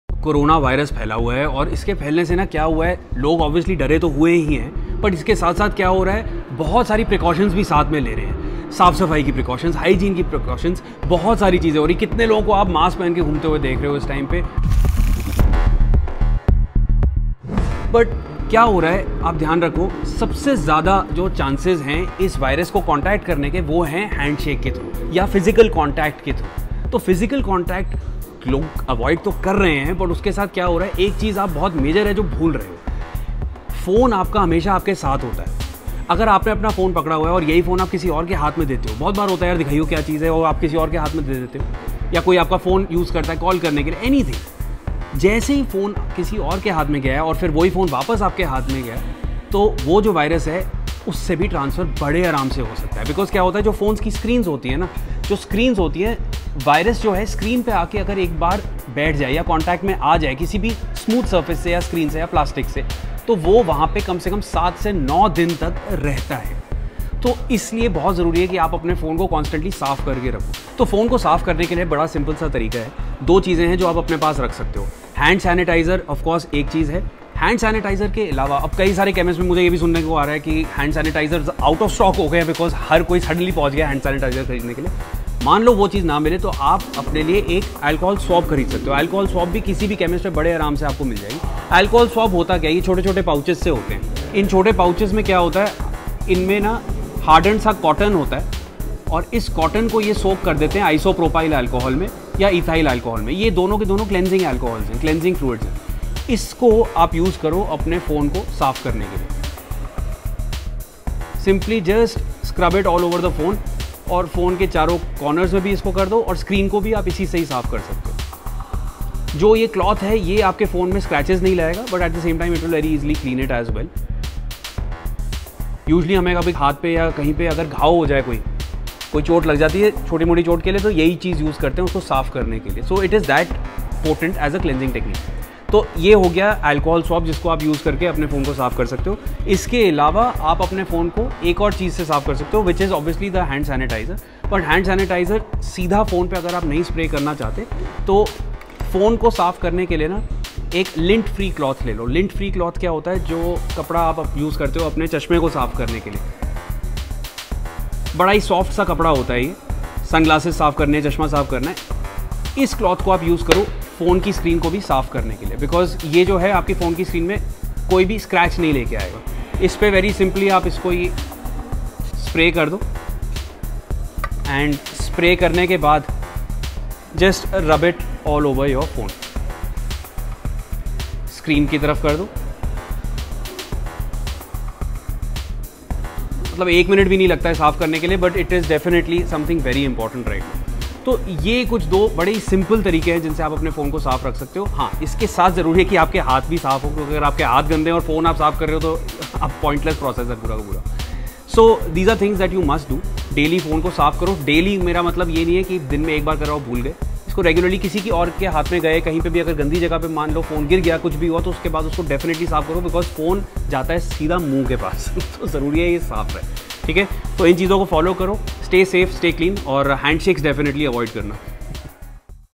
CORONAVIRUS | Why You Should Sanitize Your Phone | Tech Tak
Coronavirus has been spreading all across the world and handshakes have now become a major cause of concern.
But it is not just handshakes, it is your smartphone as well which can be a major carrier of the deadly coronavirus.
Watch this video to know how you can sanitize your phone and stay safe...